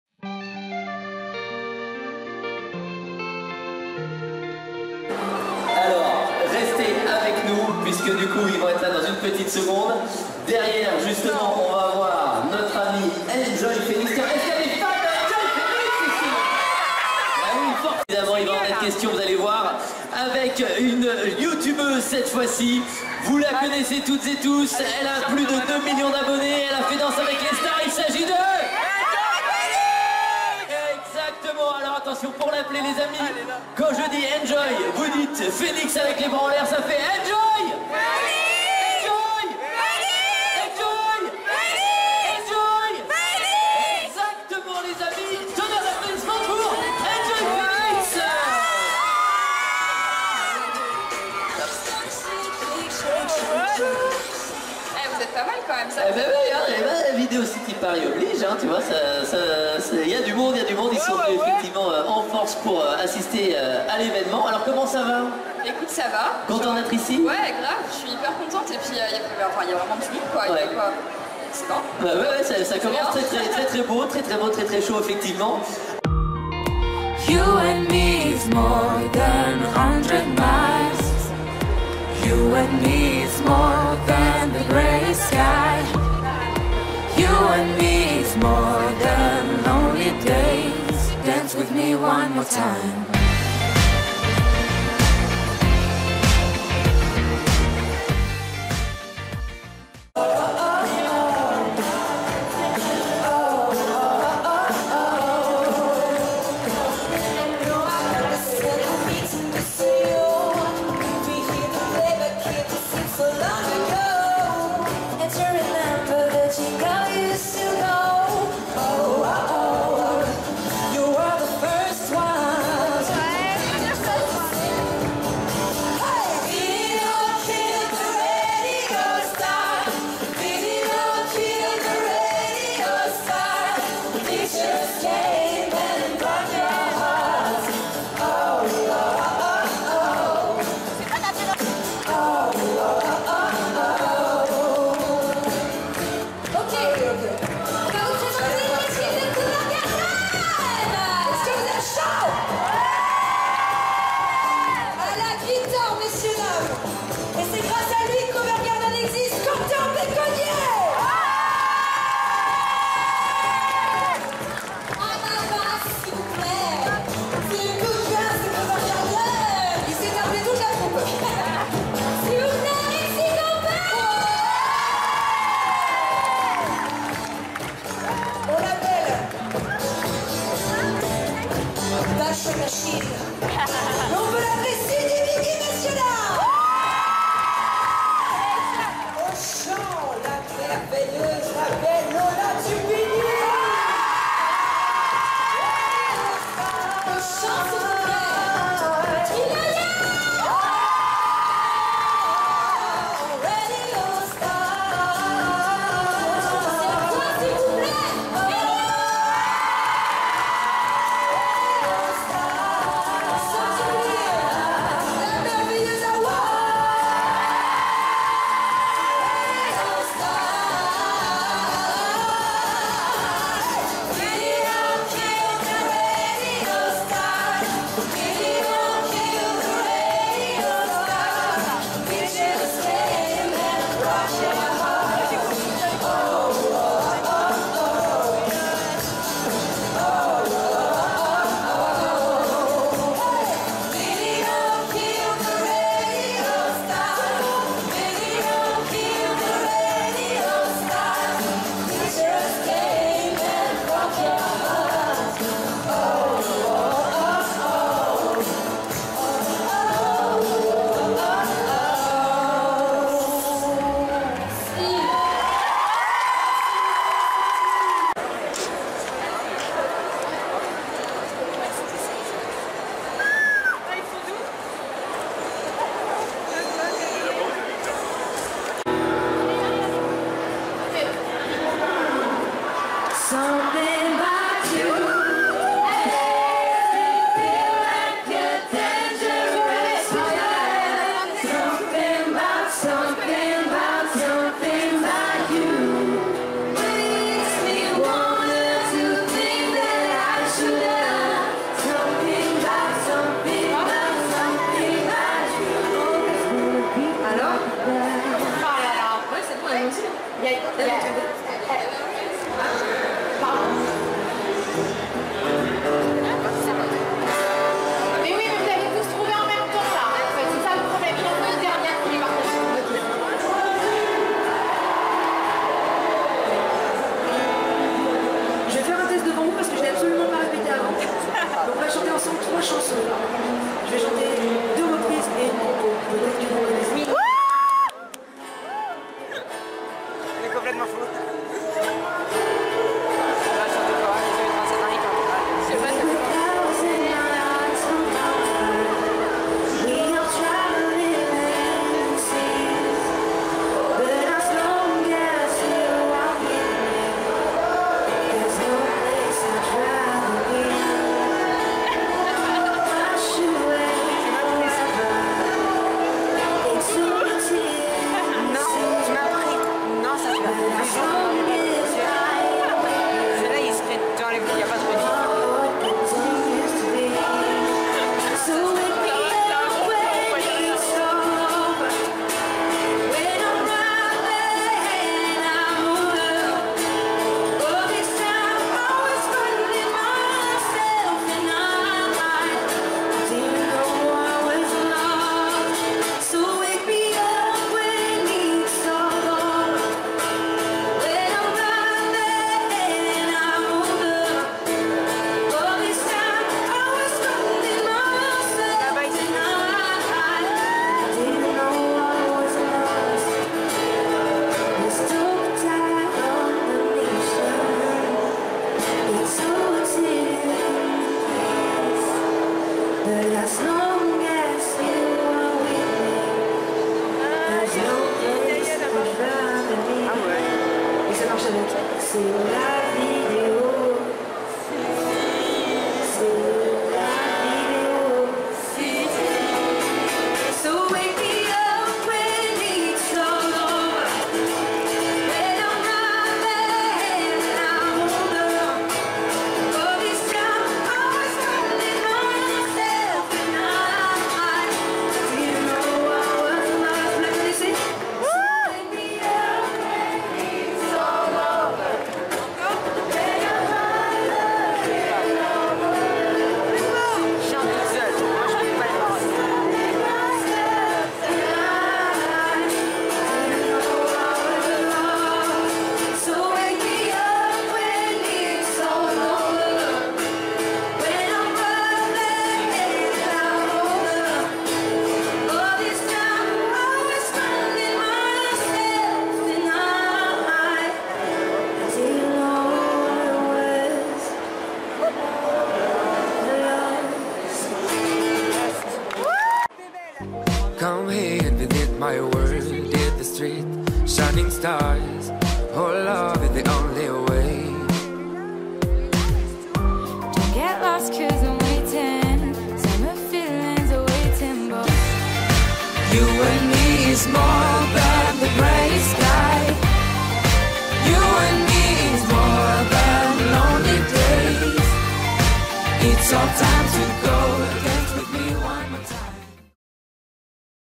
Eléa à la Vidéo City Paris 2017 !! VLOG
Salut à tous :D

Je suis tellement heureuse d'être allée à la Vidéo City Paris 2017!!! :D

Pour cette occasion, je vous ai fait un vlog donc j'espère qu'il va vous plaire :)
Si c'est le cas, likez et partagez la vidéo, dites-moi en commentaires si vous aussi y êtes allés et n'oubliez pas de vous abonner à ma chaîne si ce n'est pas encore fait!